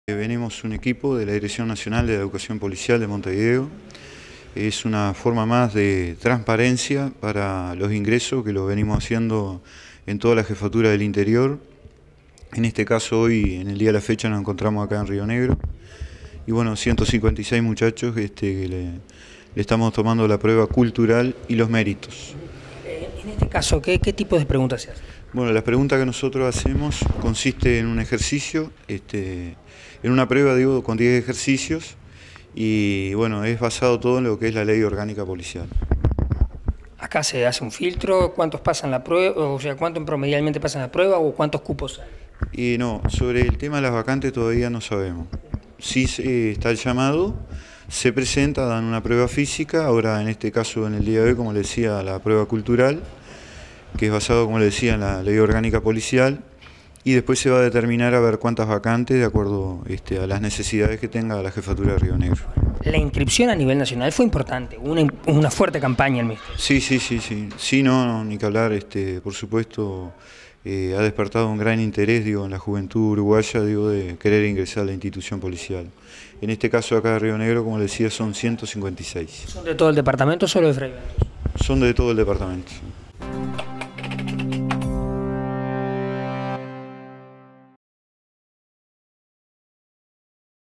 Se realizaron pruebas de ingreso a la escuela policial.
157 aspirantes en Rio Negro dieron la prueba de ingreso a la escuela policial informó el comisario Jacinto Martinez de la Dirección de Educación Policial.